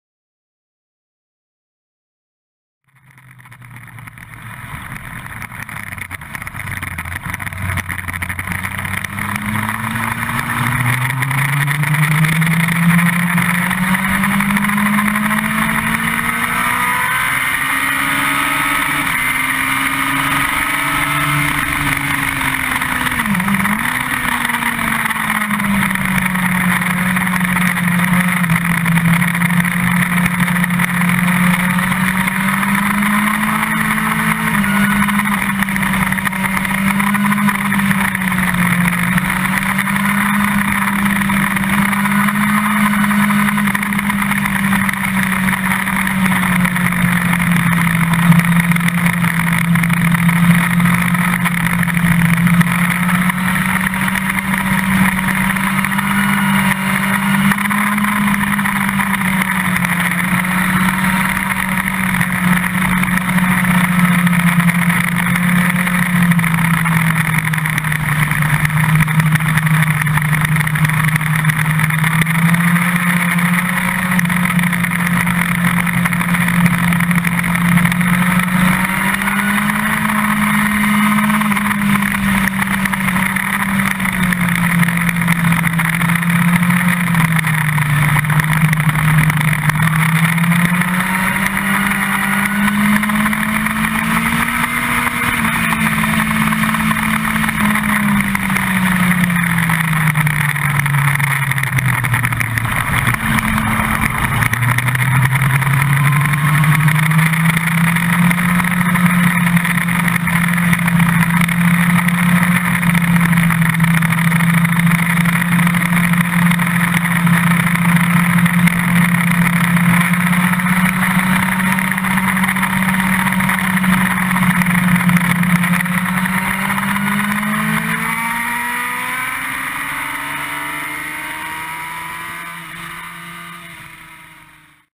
Piloto Juan Carlos Craviotto Jimenez Curso Almeria Escuela MAC
Aprendiendo y Disfrutando durante el Curso de Conducción que tuvo lugar en el Circuito de Almeria el pasado mes de Abnil.

El nivel Violeta es un nivel de Perfeccionamiento, con participantes con experiencia a los que les proponemos ejercicios para mejorar el manejo simultaneo de mandos de manera instintiva consiguiendo realicen una conducción relajada, pudiendo dedicar su atención a los factores externos que ofrece la conducción en carretera.

A medida que dominan las técnicas de conducción más básicas vamos incorporando nuevos aspectos a los que deben prestar atención para evolucionar en su conducción segura.

Muchas gracias